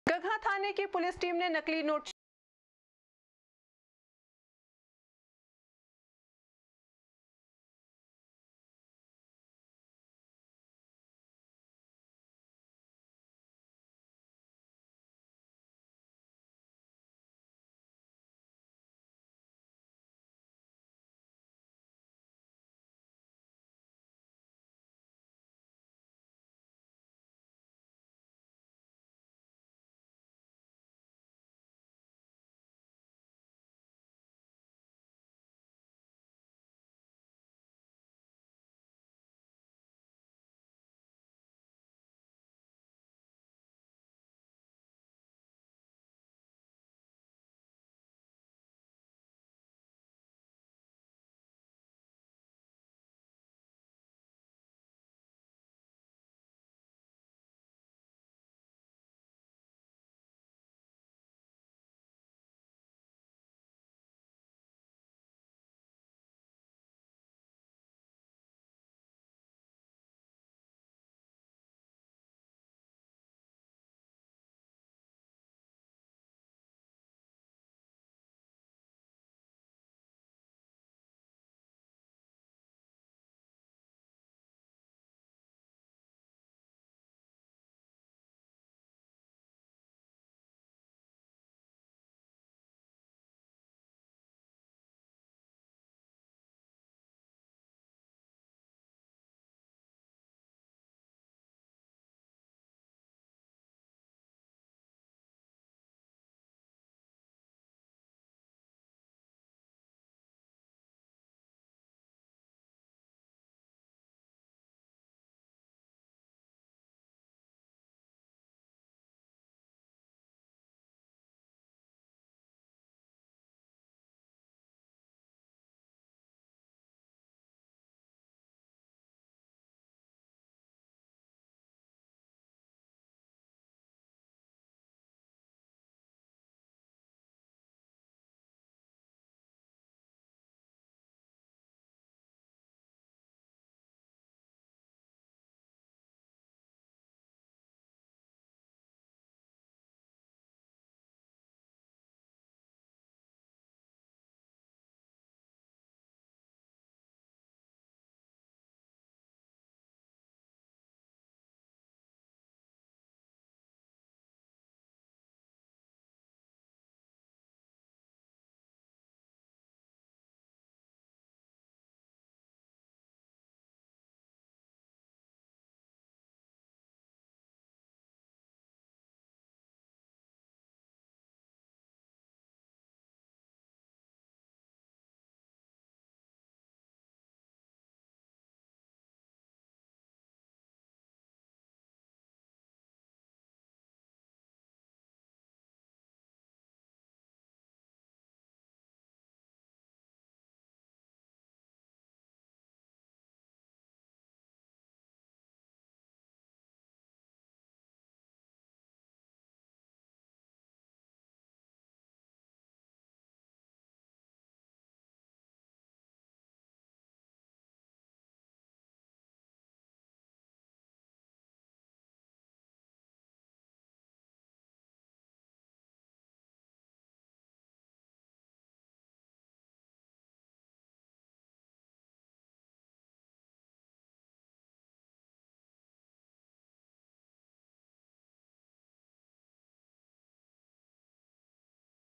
गगहा थाने की पुलिस टीम ने नकली नोट छापने के शातिर गैंग को गिरफ्तार
गगहा थाने की पुलिस टीम ने नकली नोट छापने के शातिर गैंग को गिरफ्तार कर किया सनसनीखेज खुलासा, गिरफ्तार अभियुक्तों के पास से नकली नोट बनाने की सामग्री, 10 रुपये से लेकर 200 रुपये के नकली नोट, मोबाइल और कार किया बरामद, भेजा जेल